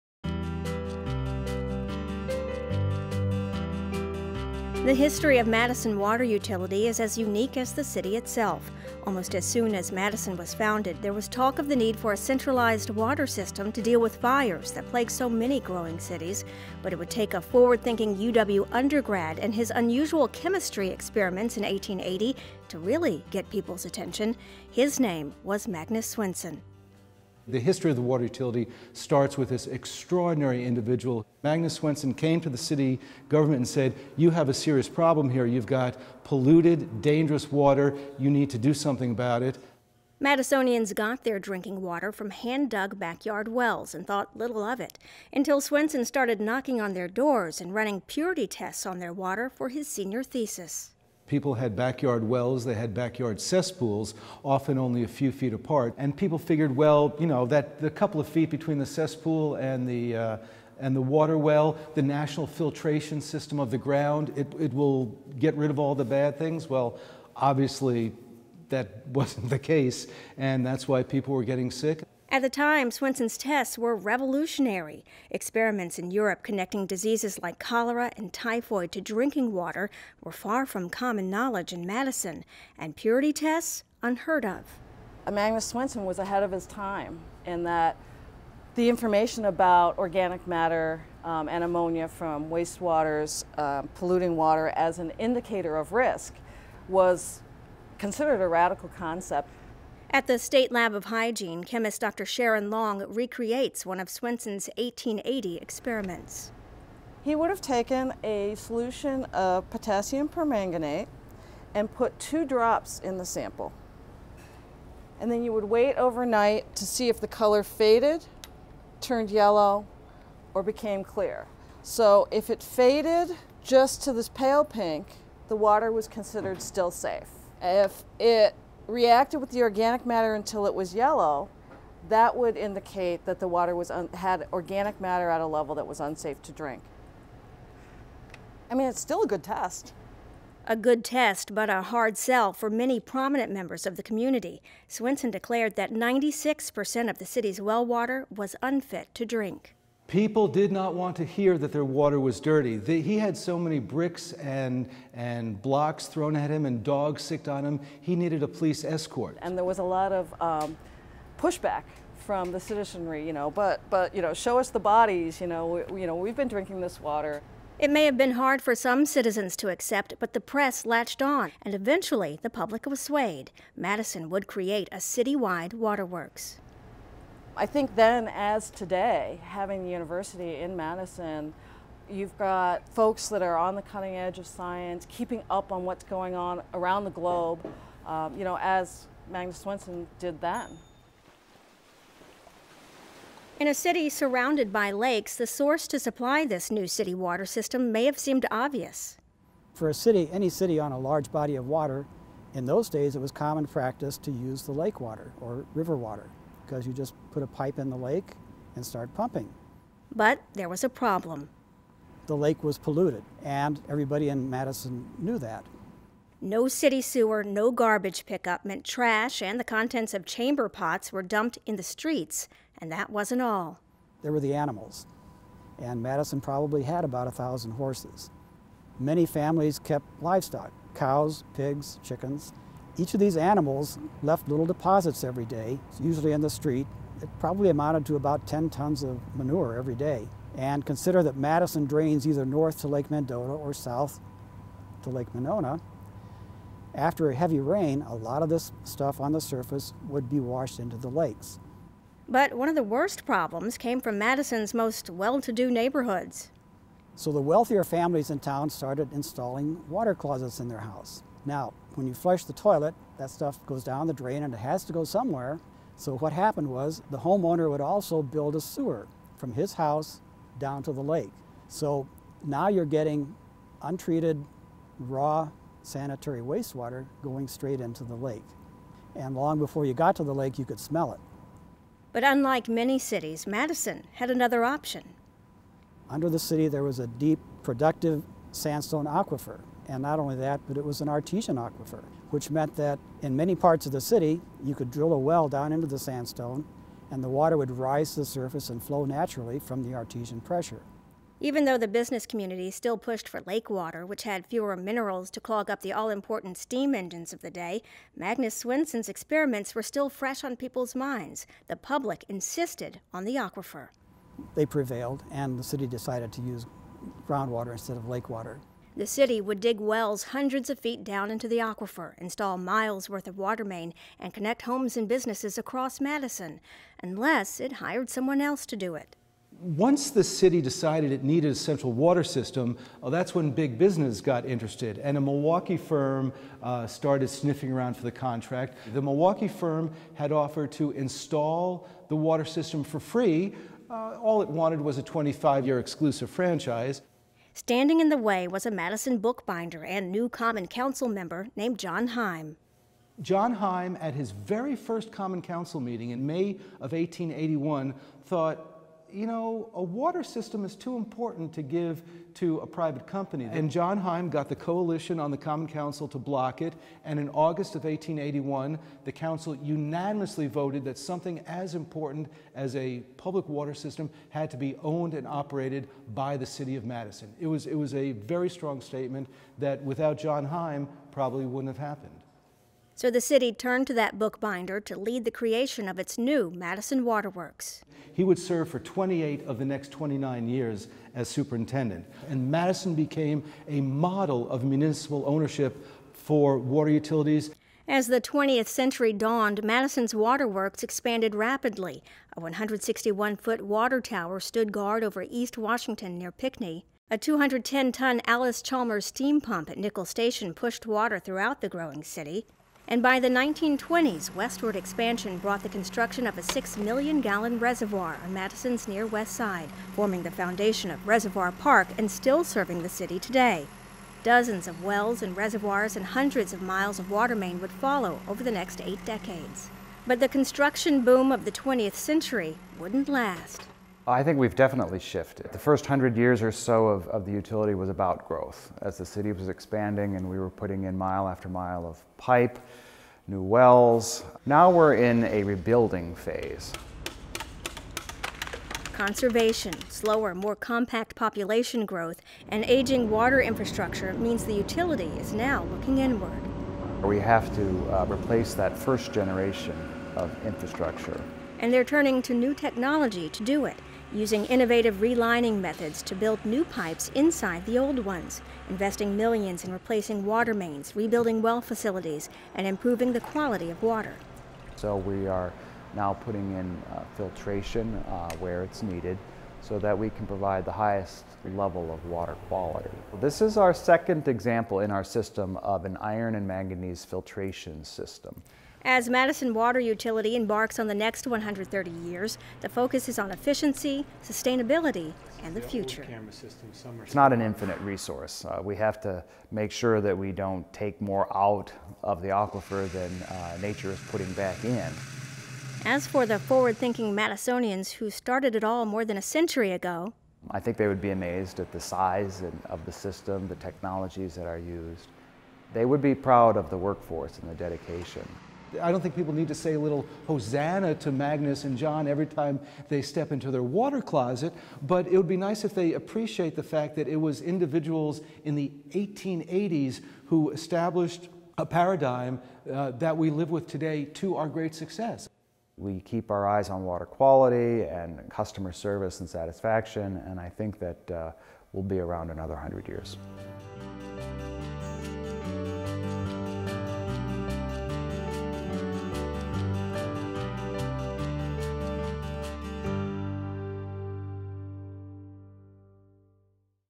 Madison Water Utility: Building on History
How did a UW undergrad's cutting-edge chemistry experiments in 1880 jumpstart Madison Water Utility? A look at the Utility's unique history and promising future.